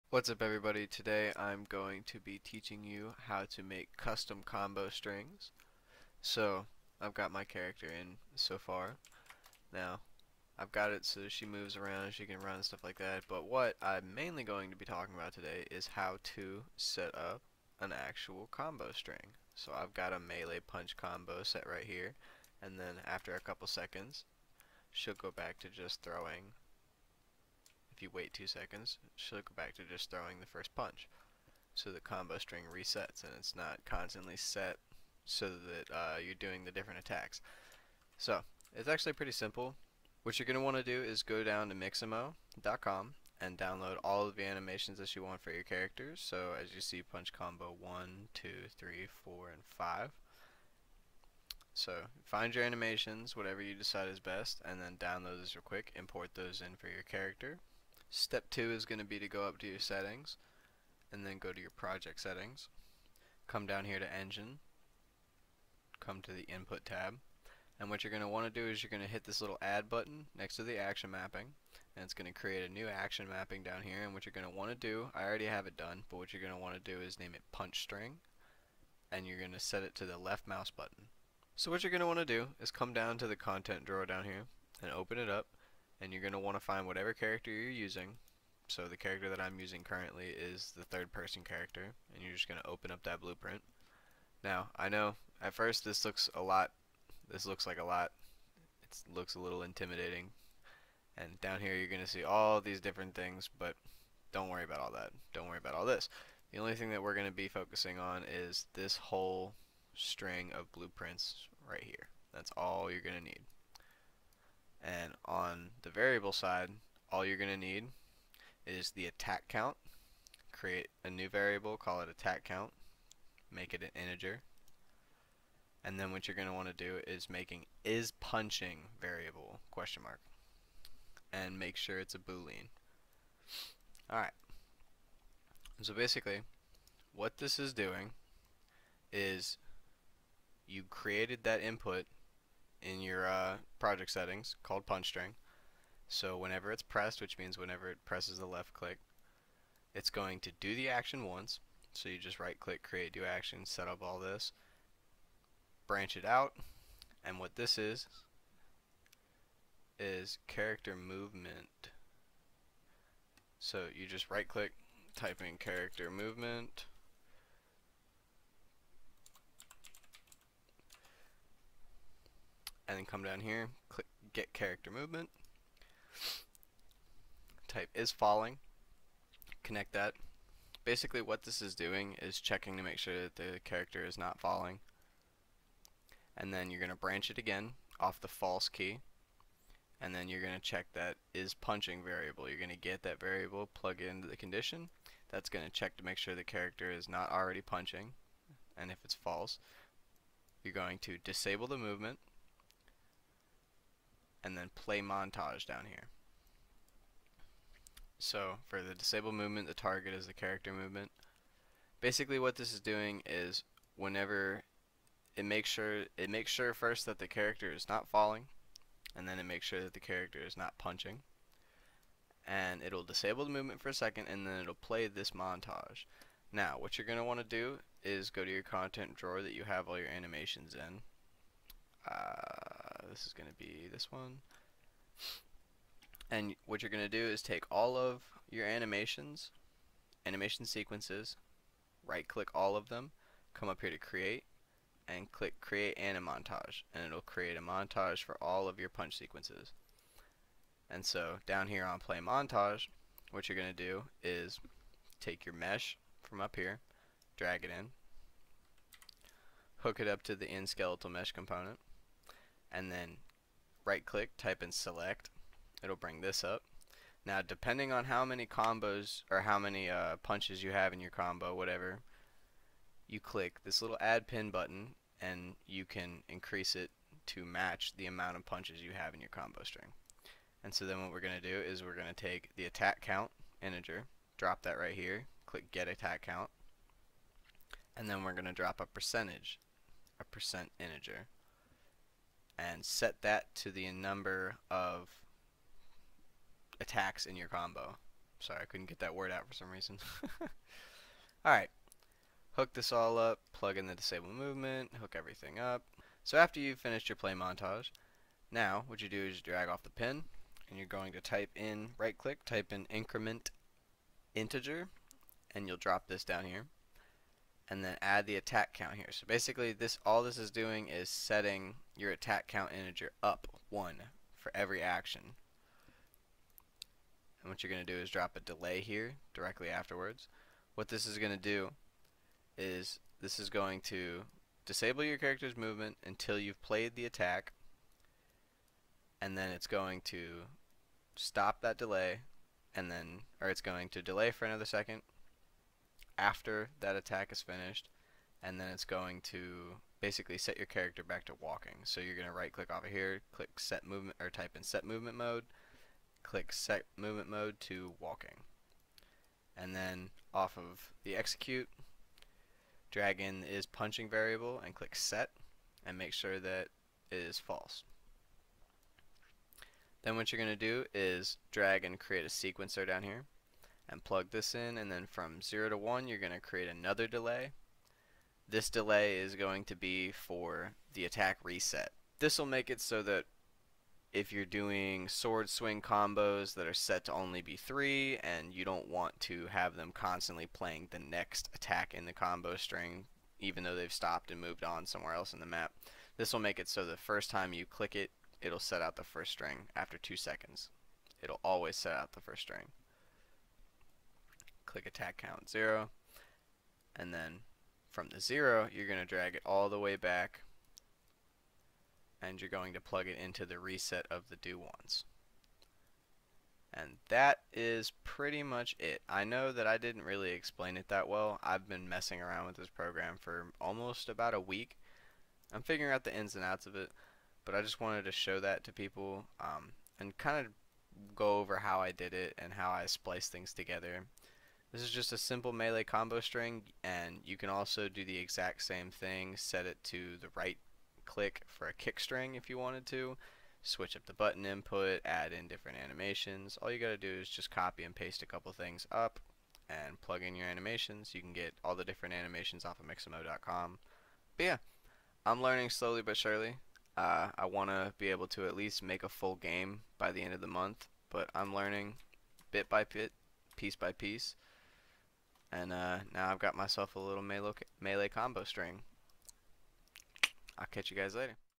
How to create a punching combo for your character in Unreal Engine 5
Today I'm teaching you guys how to import punching animations and make your own fighting combo string to use for characters in your own game!
This works with swords and other weapons, just make sure to find the animations you want to use.